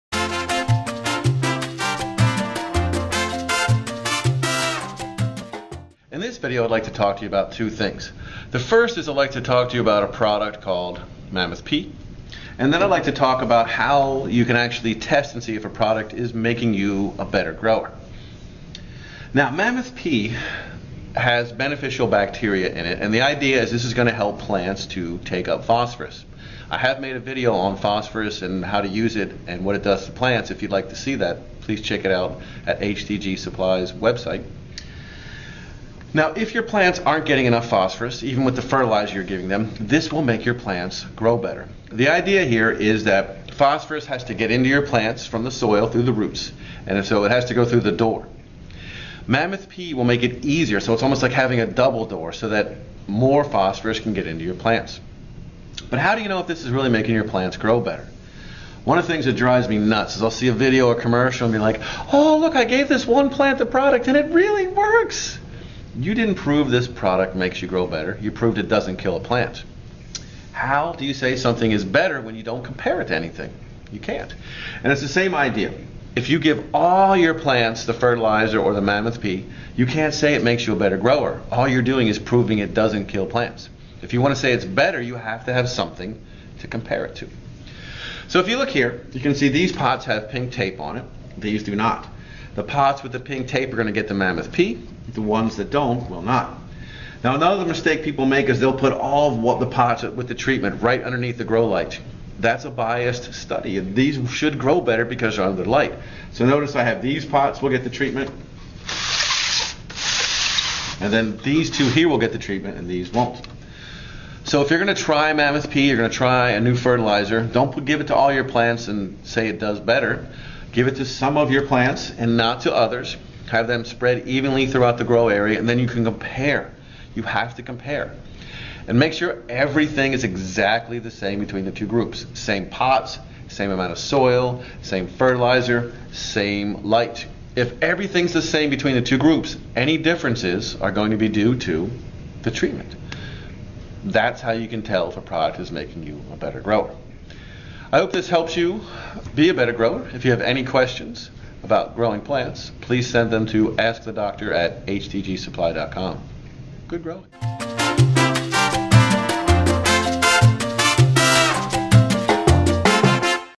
Mammoth P and Testing Nutrients in your Garden: Ask The Doc
In this video, Doc goes over the benefits of Mammoth P.
He also goes over a simple way to test your nutrients in your garden to determine whether or not the nutrients are benefitting your plants.